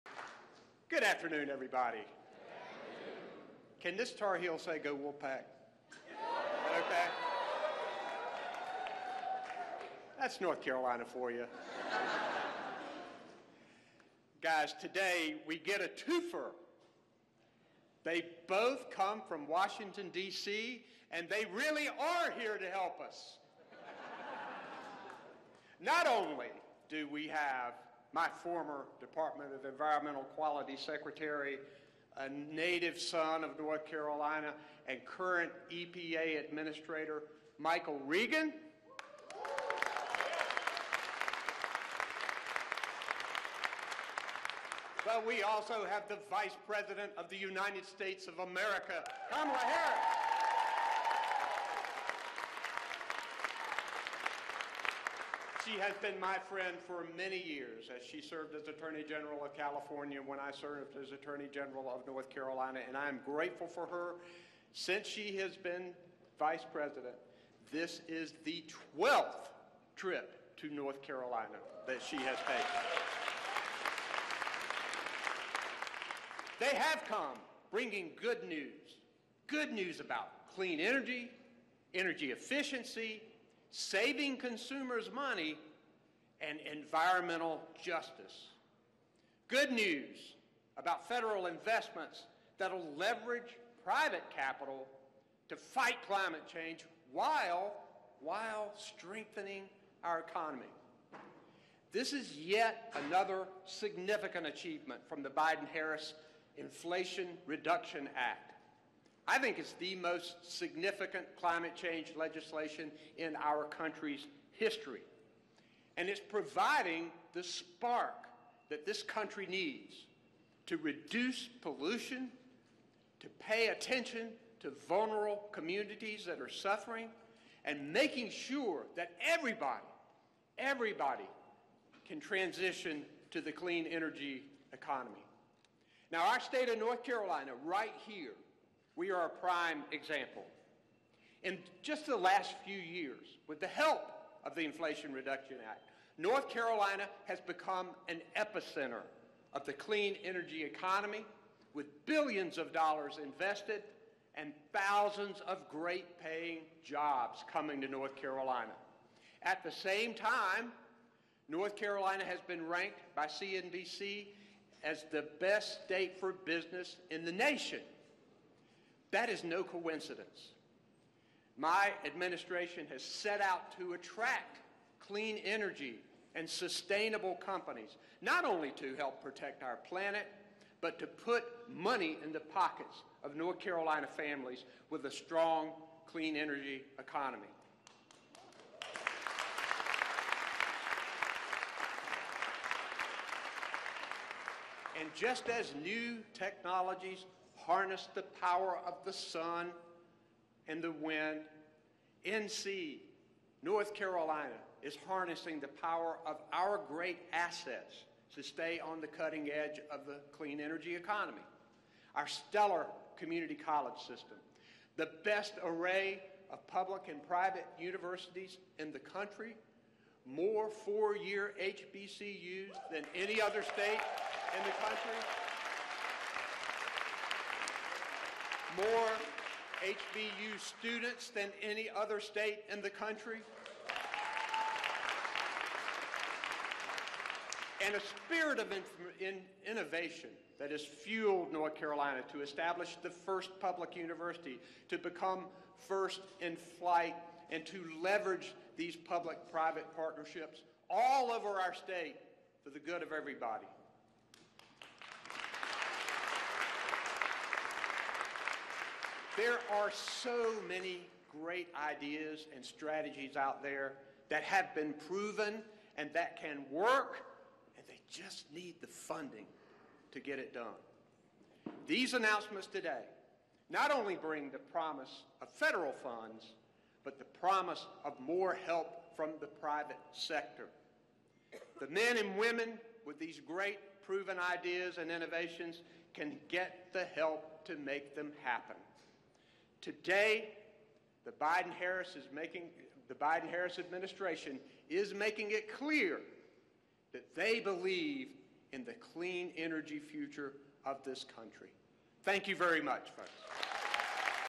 'The Most Significant Climate Change Legislation': Roy Cooper Highlights Biden Admin Bill
North Carolina Gov. Roy Cooper (D-NC) joined Vice president Kamala Harris at a climate change on event on Thursday.